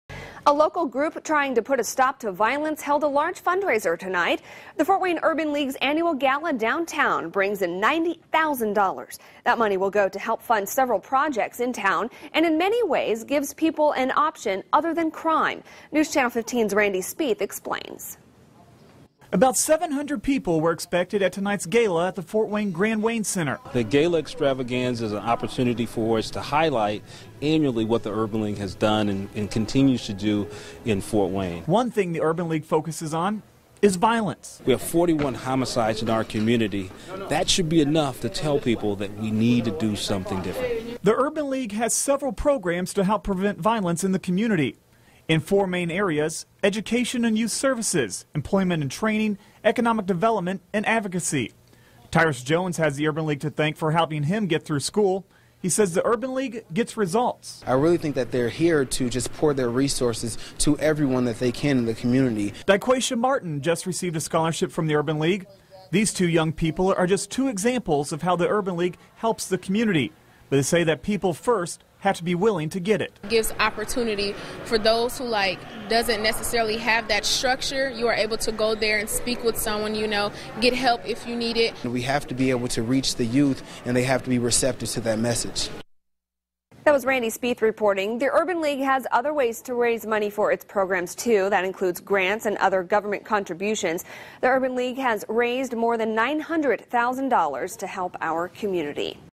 Fort Wayne Urban League hosts annual gala
Leaders with the Fort Wayne Urban League hoped Saturday's fundraiser would raise the organization close to $90,000.  The Urban League's annual gala took place this year at the Fort Wayne Grand Wayne Center.